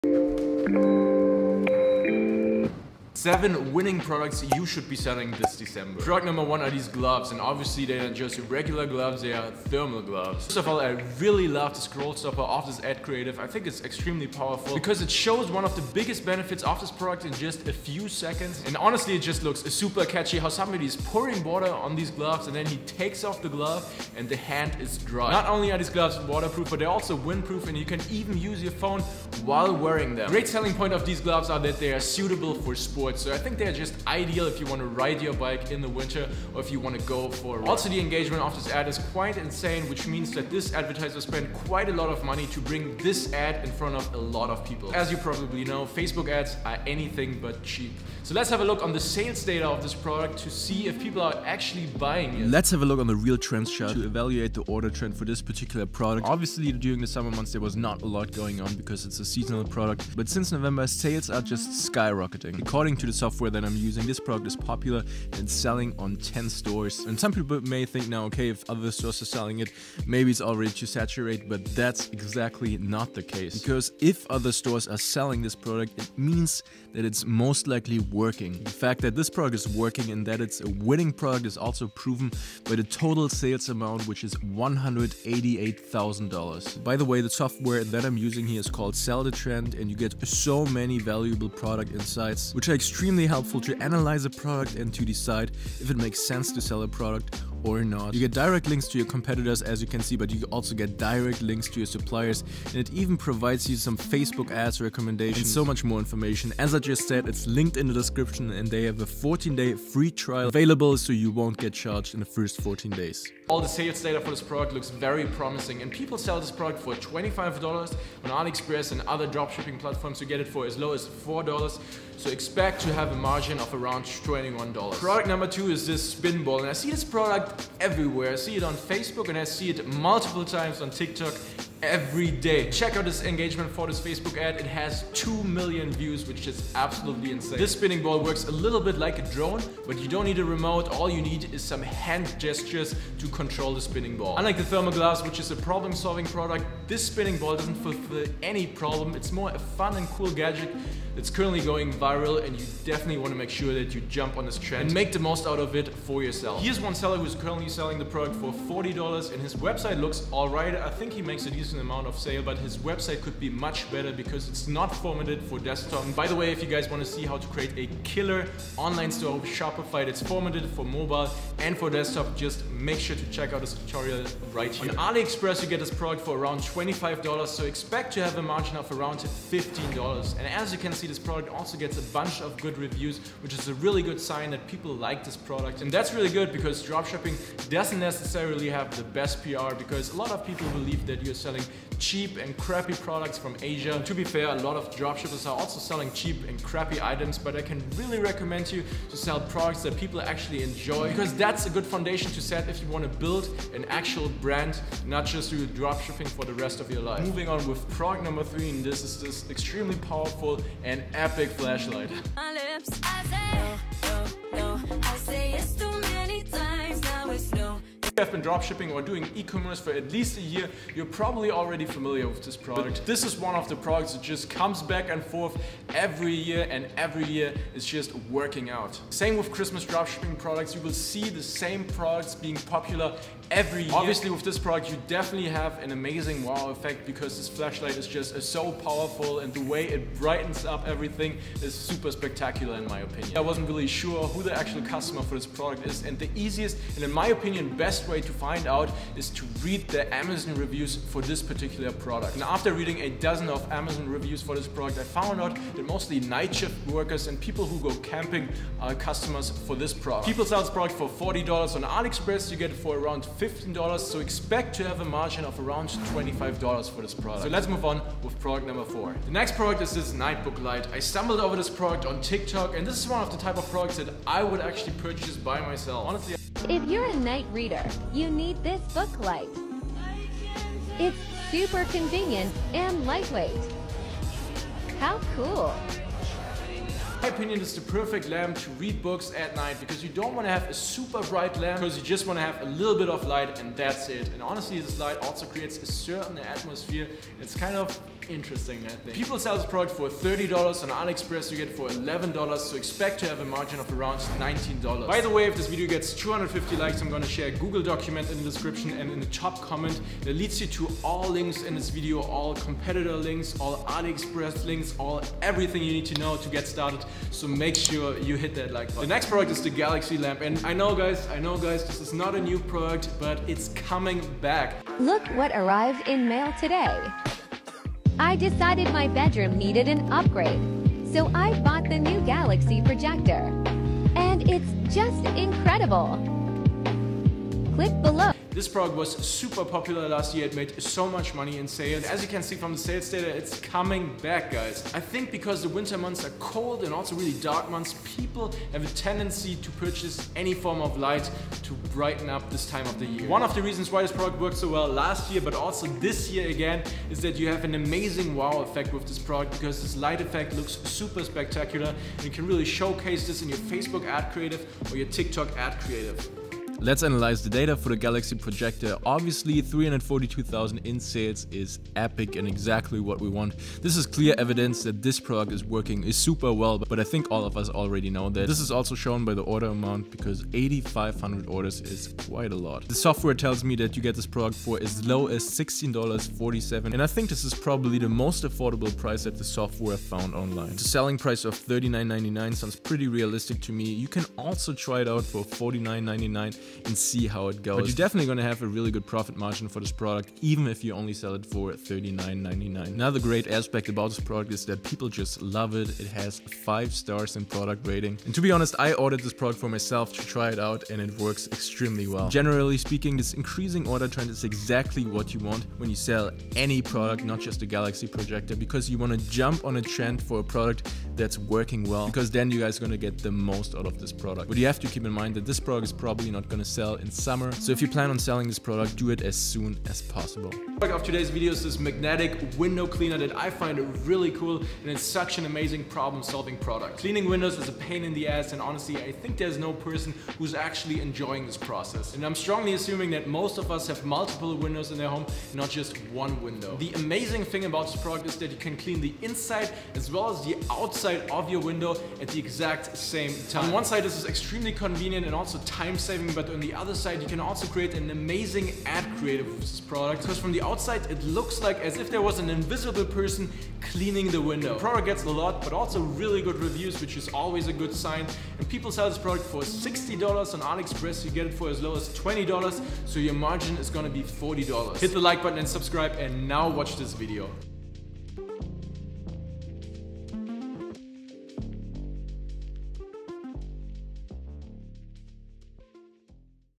TOP 7 Winning Products You Should Sell in December 2021!
Sell this now - $100k/Month WINNING PRODUCTS to Sell In December 2021| Shopify Dropshipping.

Here are my top 7 winning products for Shopify Dropshipping to sell in december 2021. All winning dropshipping Products in this video are validated winners that generate a lot of revenue every day. I picked these potential shopify Dropshipping products by myself. Let me know what you think about my winning product selection and start to sell these now :)